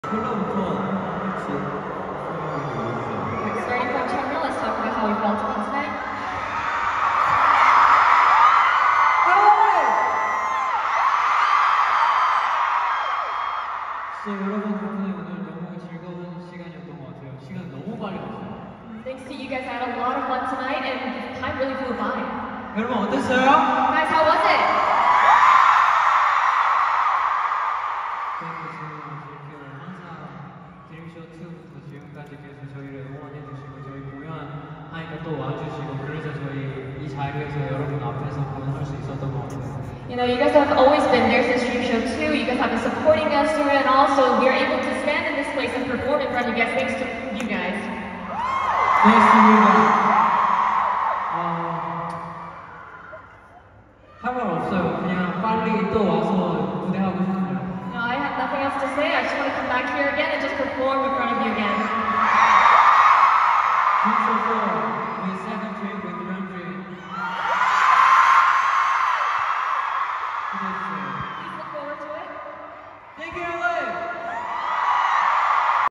NCT DREAM CHENLE (Ending ment) The Dream Show 3 in LA Los Angeles 2024
NCT Dream 엔시티 드림 Chenle (Ending ment) - The Dream Show 3 in LA Los Angeles 2024
Intuit Dome | Los Angeles, CA
12 September 2024